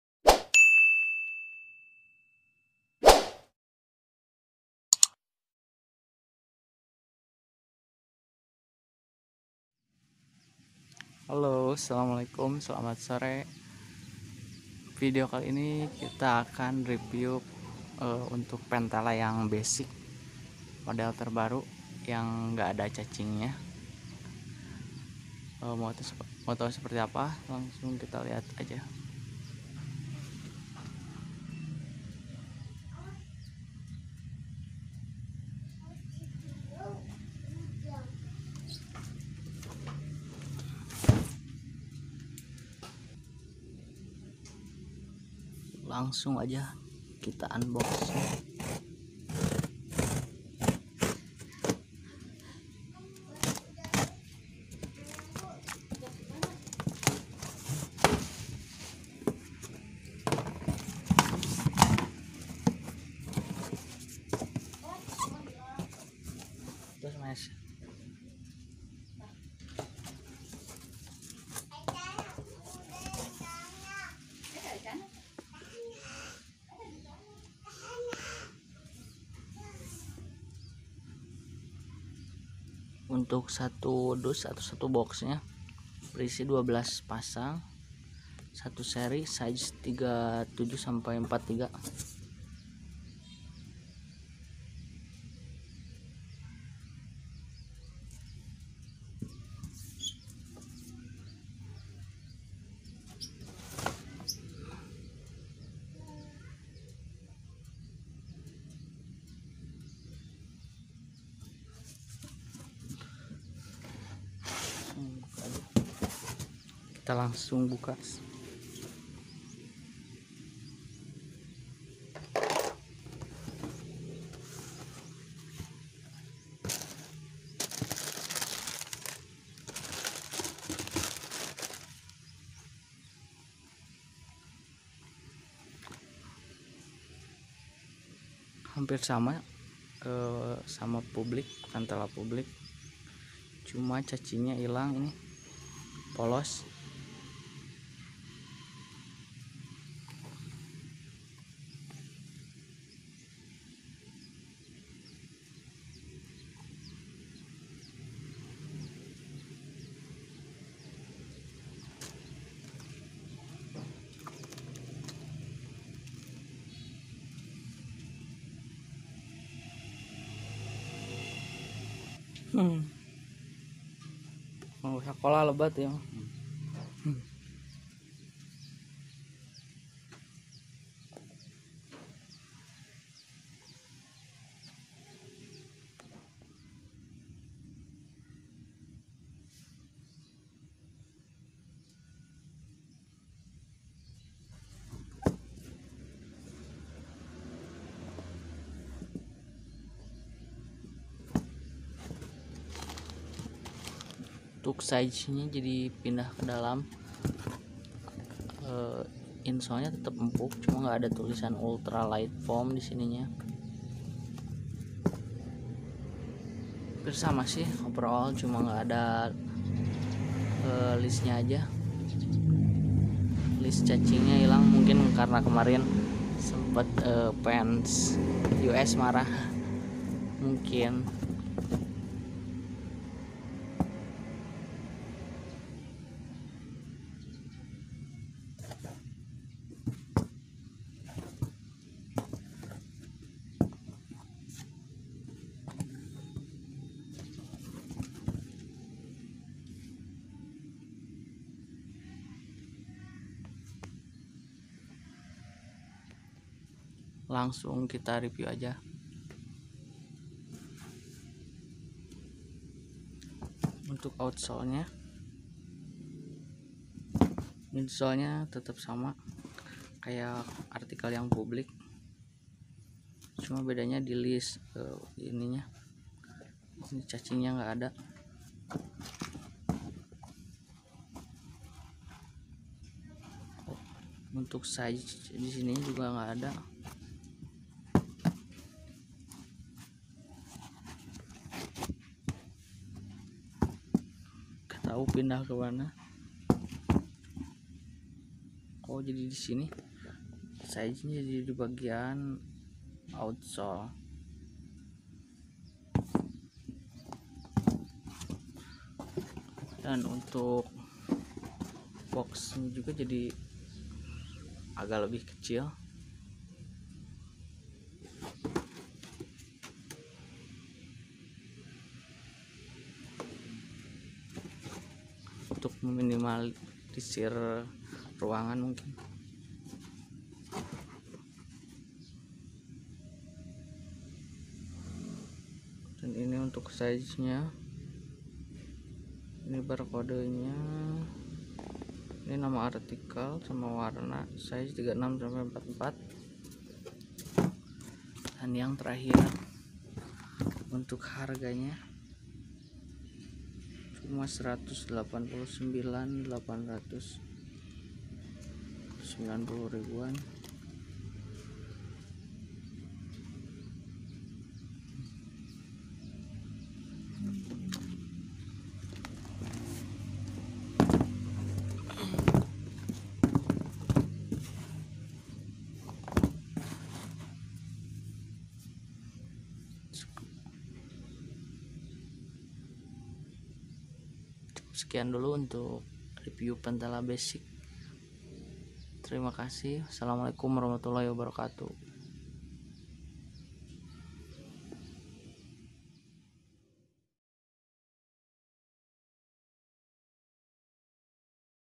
Cacing nya hilang SLURSSSS !!, Review Sepatu Ventela Basic
Unboxing Sepatu Ventela Basic Lite.

Sepatu Lokal Yang lagi Viral  , Bukan hanya Viral tapi kualitas dari sepatu Ventela juga baik dan harganya relatif terjangkau untuk semua kalangan.